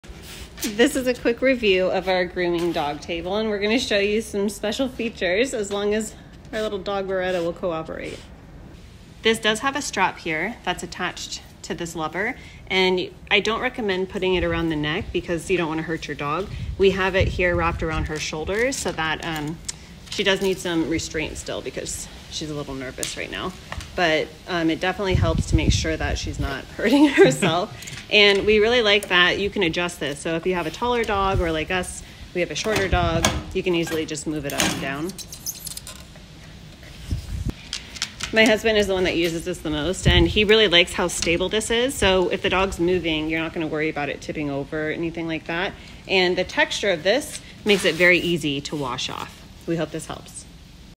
Best Dog Grooming Table For Home
We wanted to find the best dog grooming table for home so that we could save money by grooming our dogs at home. We found the Polar Aurora Dog Grooming table on Amazon and decided to check it out. See our review for our special tip on keeping your dog safe while using this table.

*I purchased this product myself.